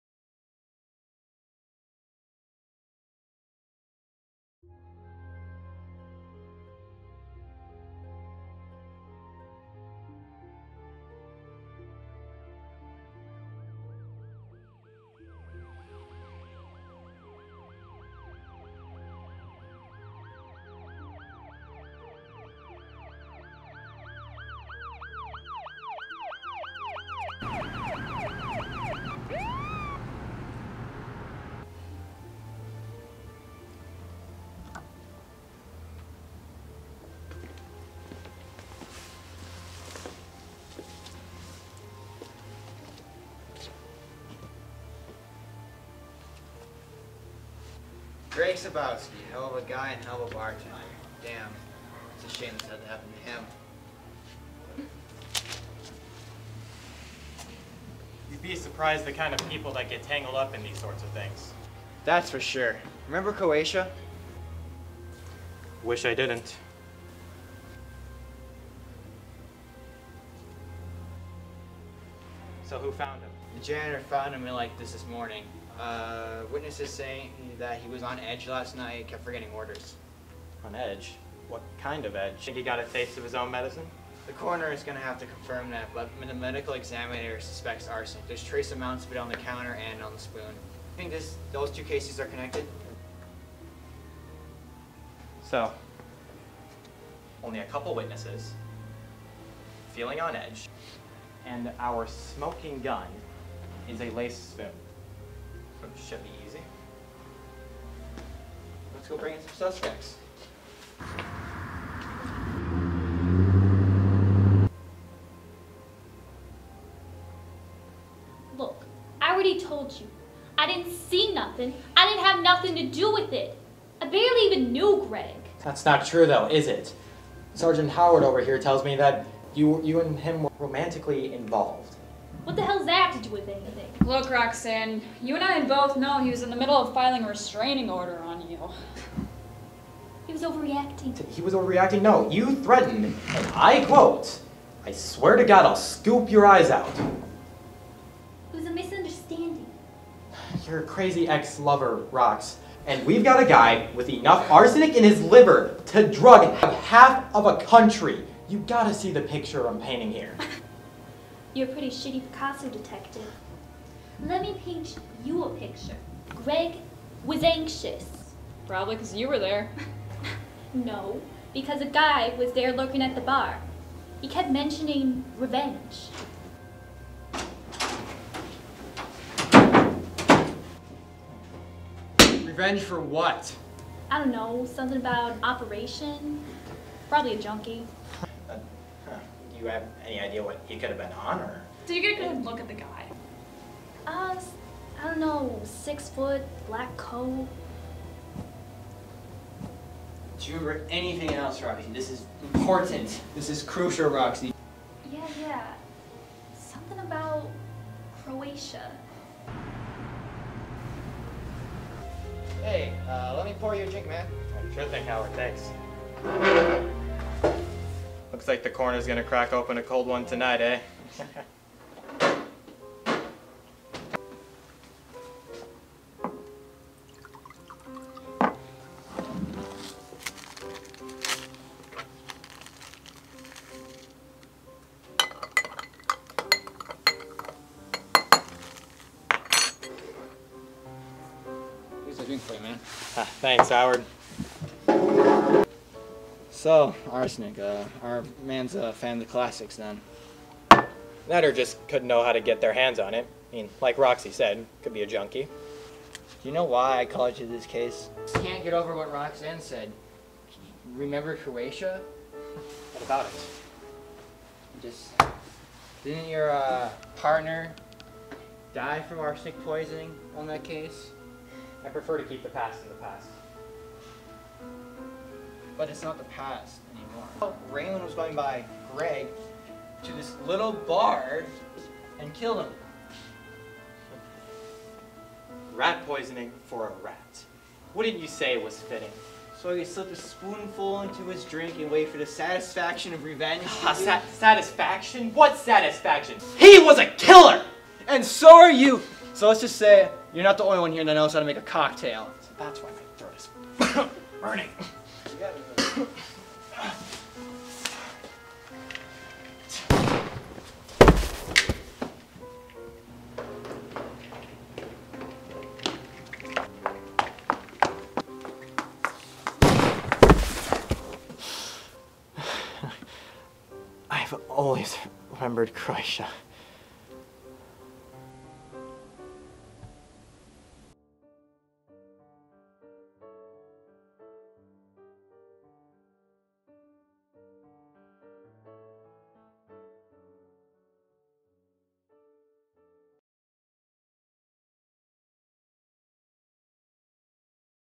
Never Forget || Short Film For UWRF 48 Hour Film Festival
"Never Forget" a film by team "We're Trying" for the annual fall 48 Hour Film Festival held by University Wisconsin River Falls

Cast and Crew Credits:

Director: Hunter Panoch

Writers: Devon Mares, Ben Brannon, Logan Carlson, and Hunter Panoch

Producers: Hunter Panoch, Devon Mares, and Ben Brannon

Lead Cast:

Logan Carlson as Private Investigator,

Collin Simone as Detective,

Devon Mares as Medical Examiner/Cop,

Adrienne Quinlan as Roxanne

Supporting Cast: Ben Brannon as Greg "the corpse" Bartender

Director of Photography: Hunter Panoch

Editor: Hunter Panoch

Props and Costuming: Logan Carlson, Hunter Panoch, Adrienne Quinlan, Devon Mares, and Collin Simone

And with a special thanks to Devon Mares for helping us with everything during this competition, it was amazing getting to work with you again!